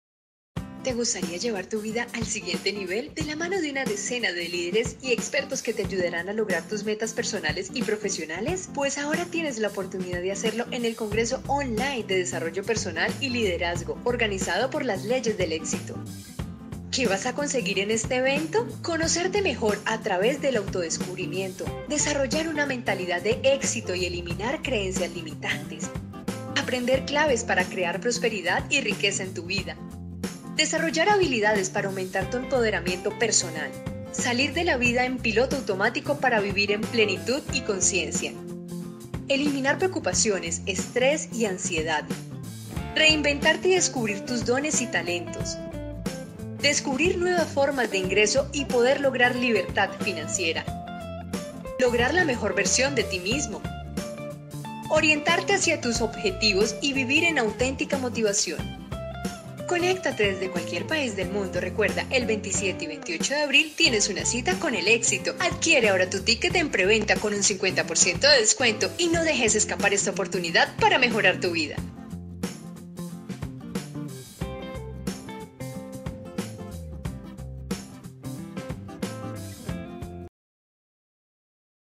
Congreso online de Desarrollo personal y Liderazgo
En el Congreso online de Desarrollo Personal y Liderazgo, participan 11 ponentes expertos, que te ayudarán a llevar tu vida al siguiente nivel, empoderándote en las distintas áreas de la existencia.
Colaboran: Global Alumni, Abueling, Ediciones Obelisco, Imperio Digital, Leadgods.